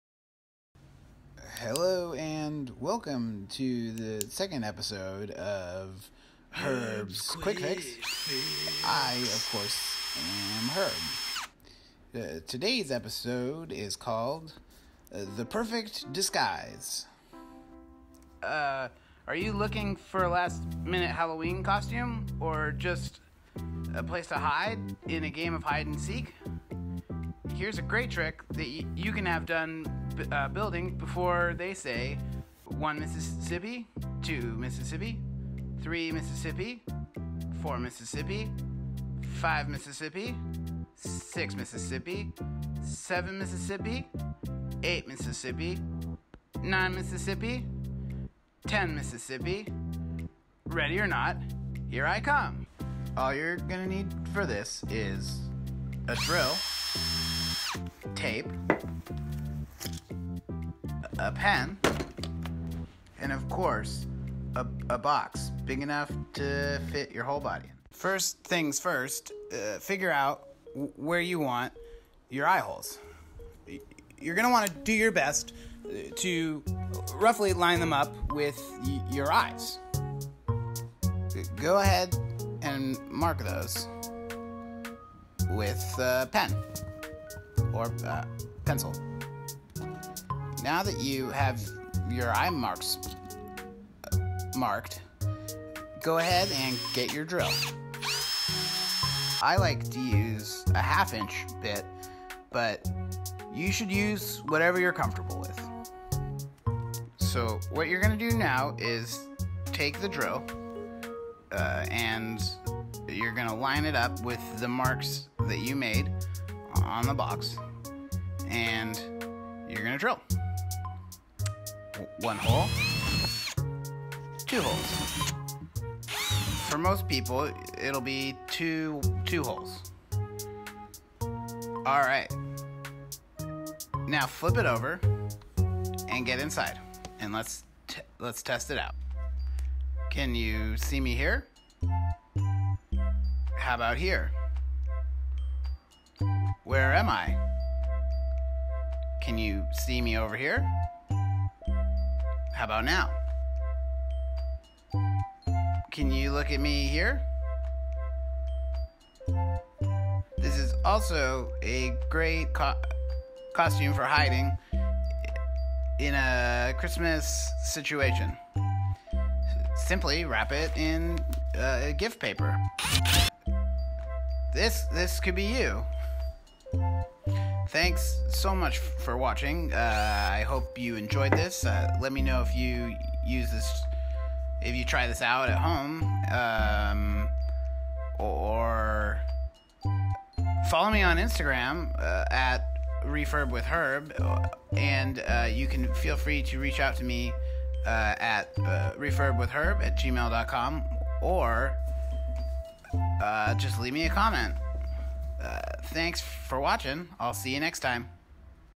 Herbs Quick Fix Episode Two: Perfect Disgise
Hello my friends! I am very excited to present the second episode of "Herb's Quick Fix"! In This episode I show off a pretty sweet (and budget friendly) costume/ hiding spot Idea.